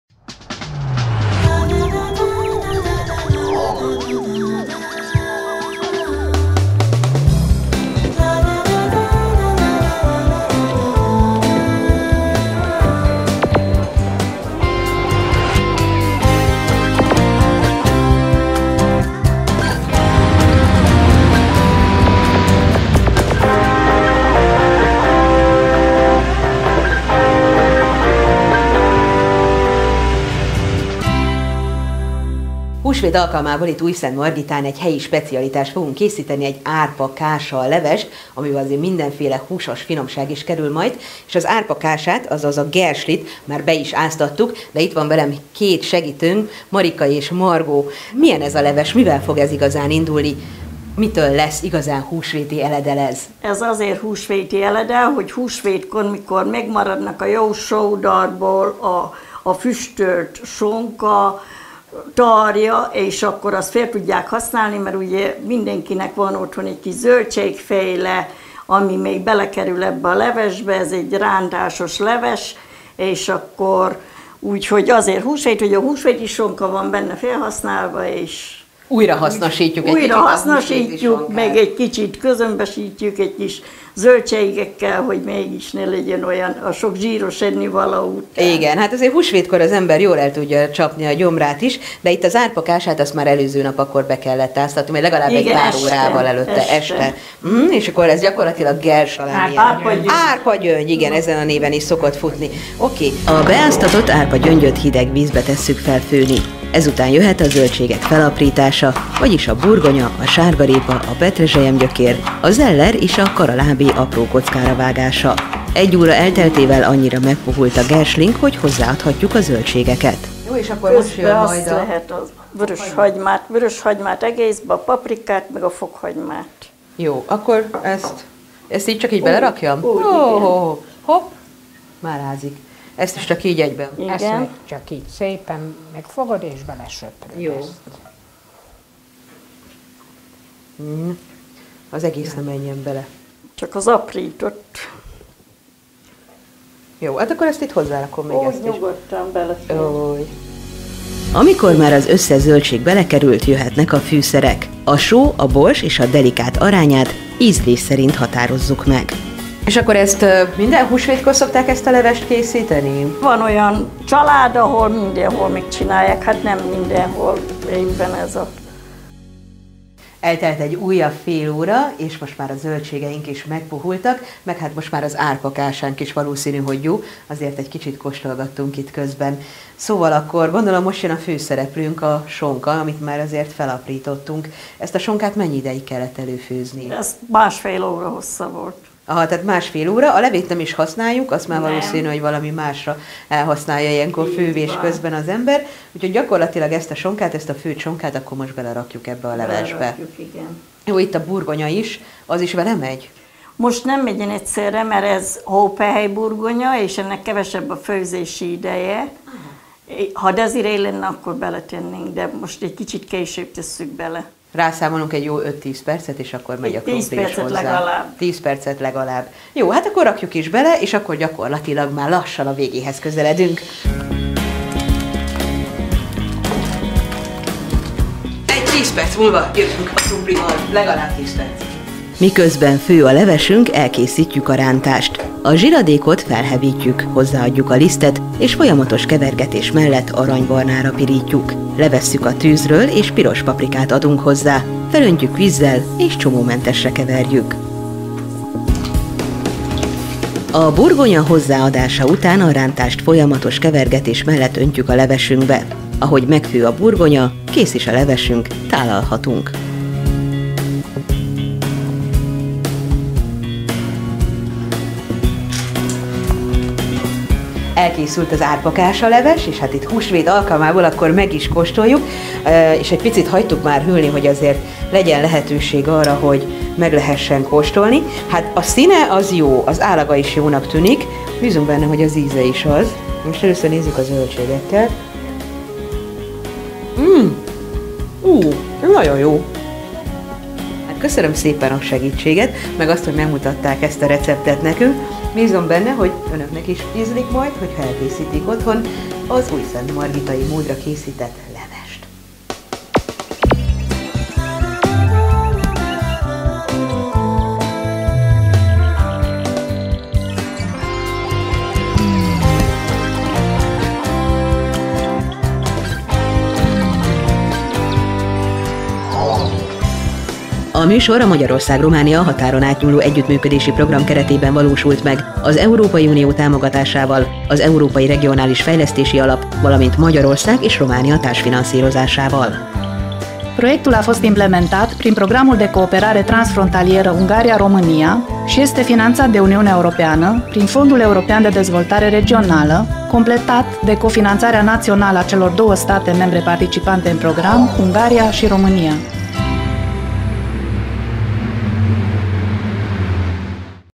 Kapocs Gasztro - Árpakása Leves Húsvétra (Újszentmargita)
Húsvétkor jellemzően sokat eszünk A főtt sonka levéből és a megmaradt húsból akár egy igen ízletes levest is készíthetünk. Újszentmargitán hagyományosan gerslivel, azaz árpakásával készítik az ételeket. Íme egy könnyen elkészíthető húsvéti leves.

A beáztatott árpakását hideg vízbe tesszük fel főni. Ezután jöhet a zöldségek felaprítása, vagyis a burgonya, a sárgarépa, a petrezselyem gyökér, a zeller és a karalábé apró kockára vágása. Egy óra elteltével annyira megpuhult a gerslink, hogy hozzáadhatjuk a zöldségeket. Hozzáadjuk a vöröshagymát egészben, paprikát és fokhagymát. Amikor már az összes zöldség belekerült, jöhetnek a fűszerek. A só, a bors és a delikát arányát ízlés szerint határozzuk meg. Mikor megpuhultak a zöldségeink is, jöhet a sonka, 15-20 perc elteltével pedig a burgonya is a levesbe kerül.
Miközben fő a levesünk, elkészítjük a rántást. A zsiradékot felhevítjük, hozzáadjuk a lisztet, és folyamatos kevergetés mellett arany barnára pirítjuk. Levesszük a tűzről és pirospaprikát adunk hozzá. Felöntjük vízzel és csomómentesre keverjük. A burgonya hozzáadása után, a rántást folyamatos kevergetés mellett öntjük a levesünkbe. Ahogy megfő a burgonya, kész is a levesünk, tálalhatunk.

Hozzávalók:
15 dkg árpkása
20 dkg burgonya
2 szál sárgarépa
2 szál petrezselyem gyökér
negyed zeller
negyed karalábé
1 darab hagyma
2 cikk fokhagyma
1 darab paprika
só, bors, delikát (ízlés szerint)
32 dkg füstölt sonka vagy tarja
rántáshoz: 2 e.k liszt, egy púpozott e.k zsír, pirospaprika